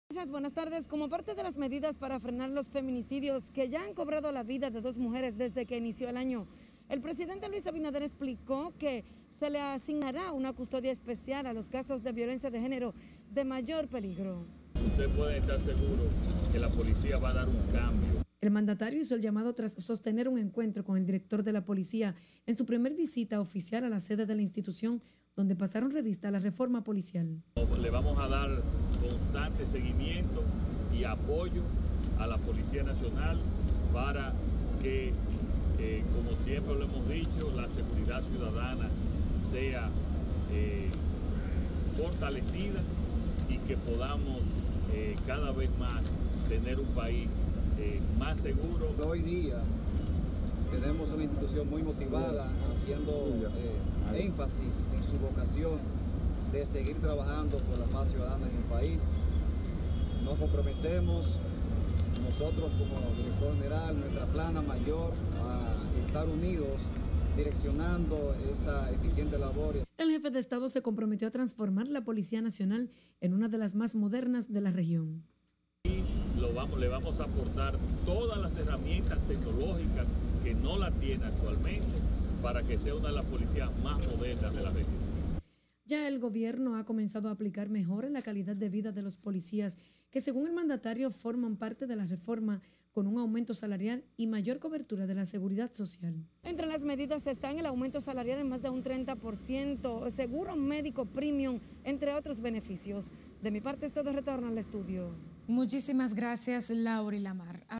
Abinader visita Palacio de la PN
El presidente Luis Abinader anunció hoy la creación de un cuerpo especializado para prevenir crímenes de violencia de género e intrafamiliar.